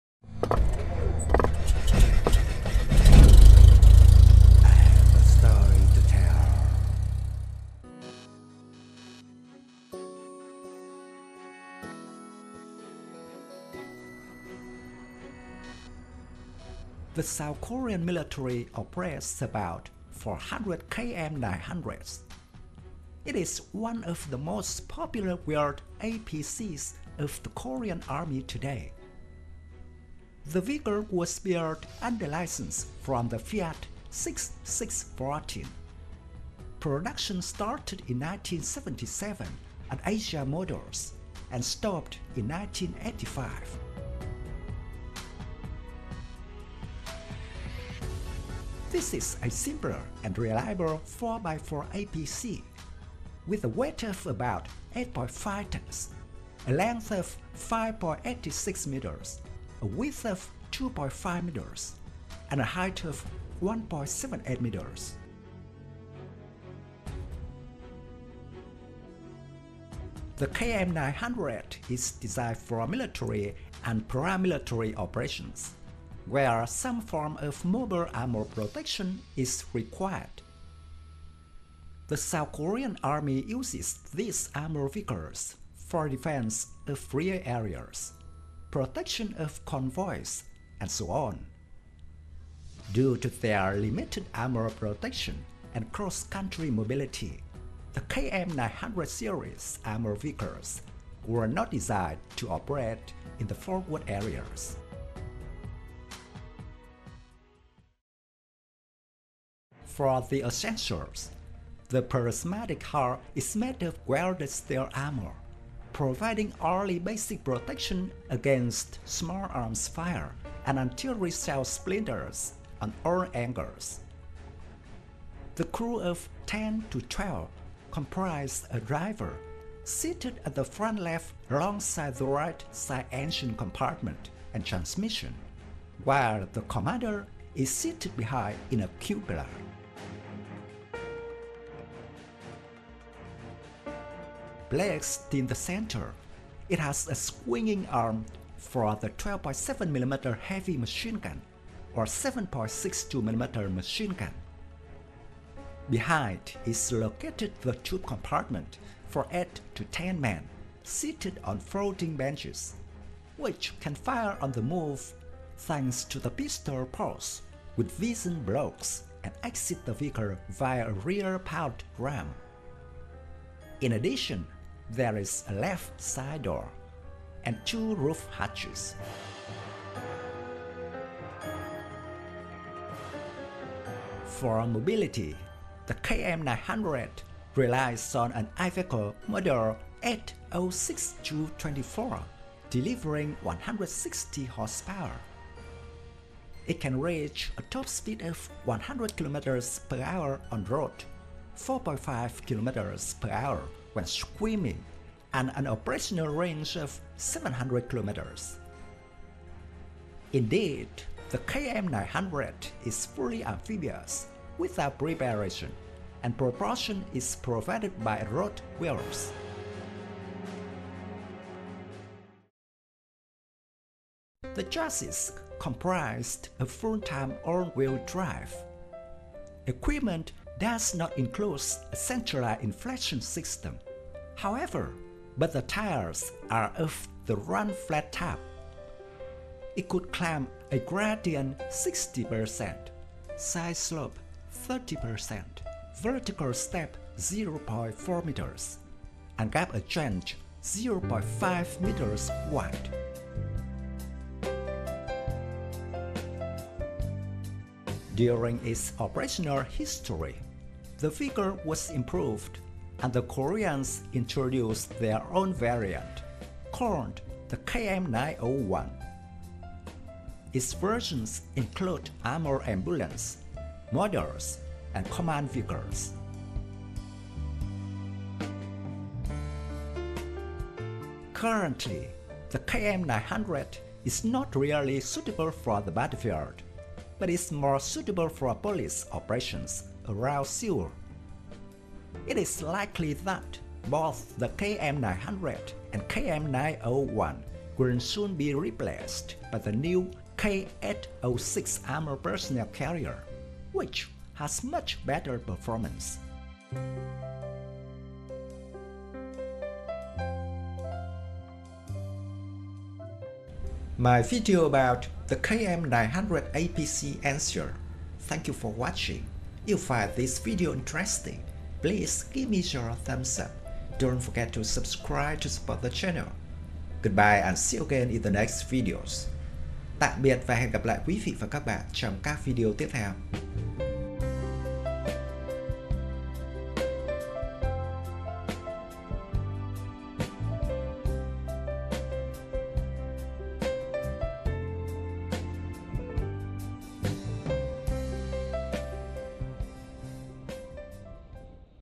KM-900 APC: The Premise For the Korean Defense Industry
The South Korean military operates about 400 KM-900s. It is one of the most popular wheeled APCs of the Korean Army today. The vehicle was built under licence from the the Fiat 6614. Production started in 1977 at Asia Motors and stopped in 1985.
KM-900 APC is a simple and reliable 4x4 APC, with a weight of about 8.5 tons, a length of 5.86 m, a width of 2.5 m, and a height of 1.78 m.

Thank you very much.
Best regards
Dung Tran.